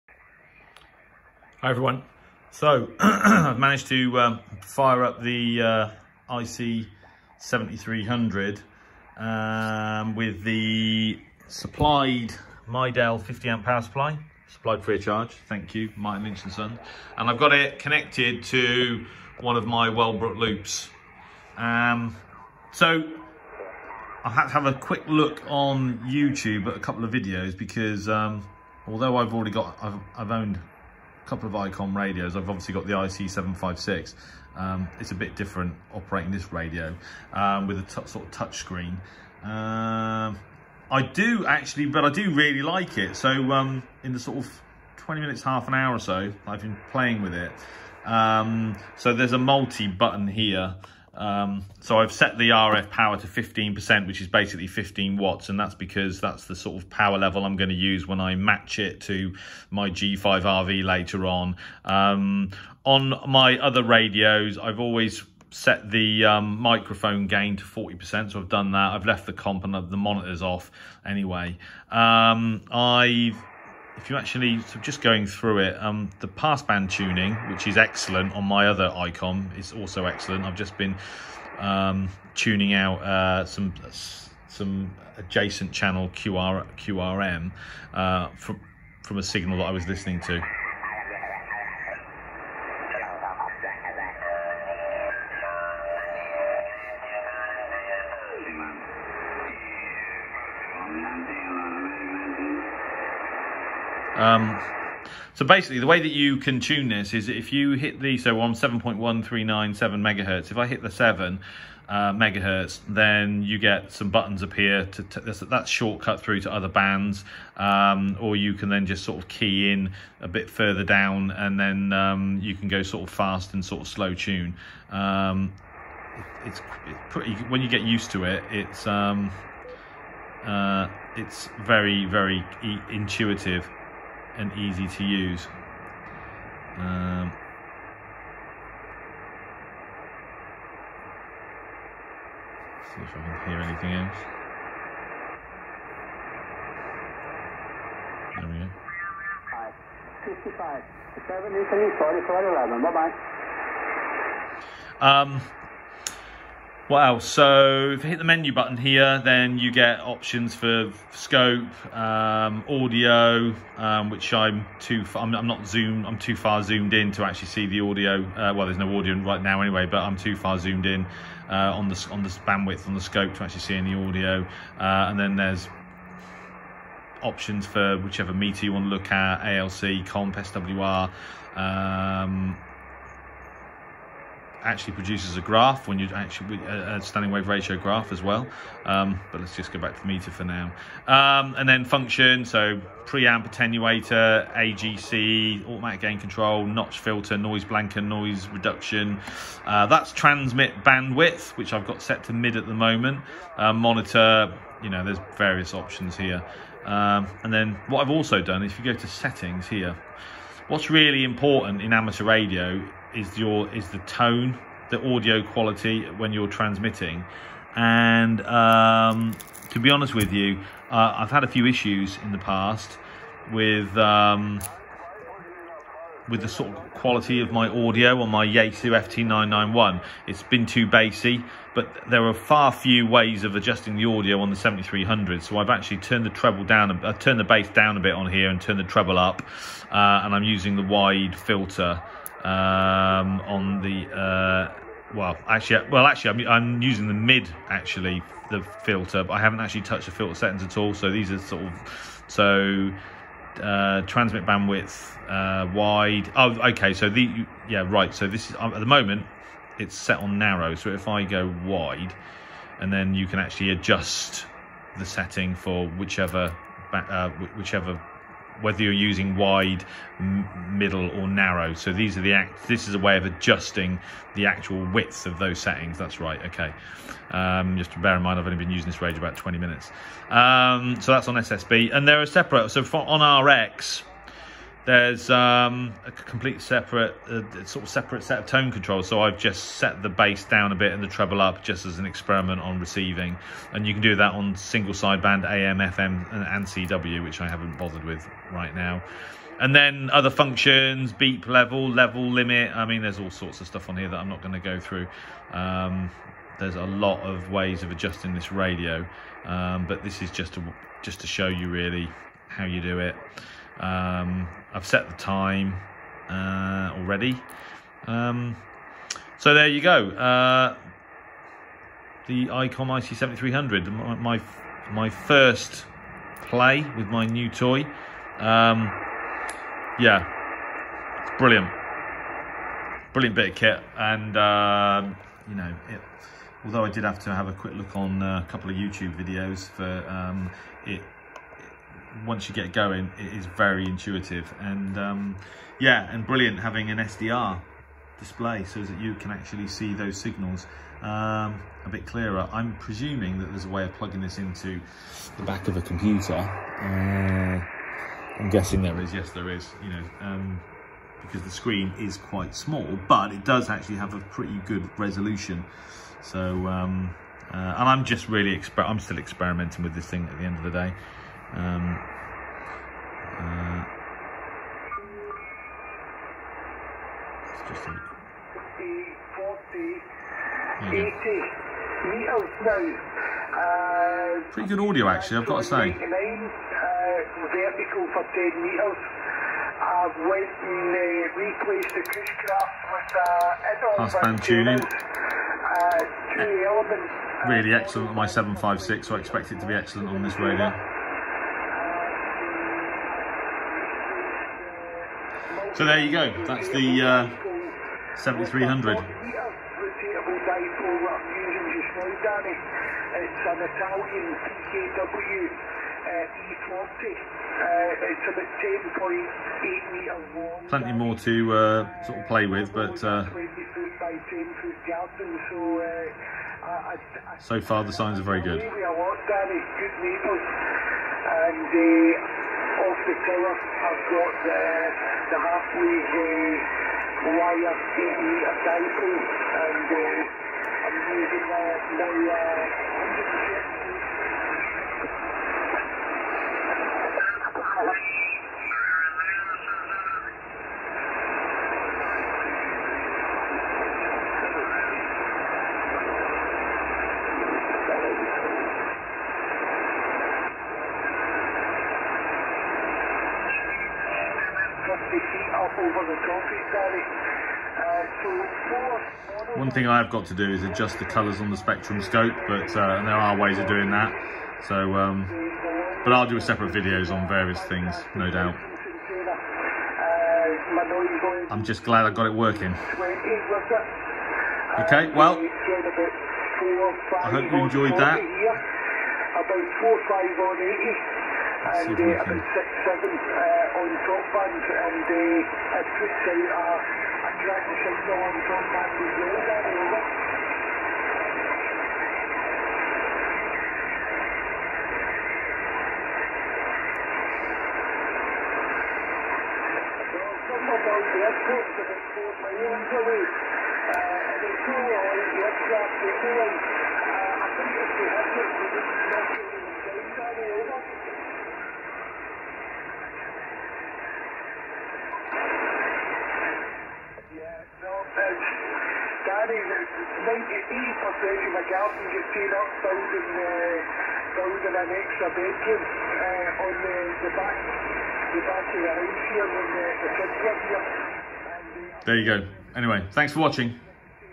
Icom IC-7300 ham radio HF/6m transceiver - first power up and a brief overview
Hi there, so I finally got the 7300 powered up for the first time. Lots to learn and settings to figure out, but already it's starting to feel quite intuitive.

Keep safe.

73s,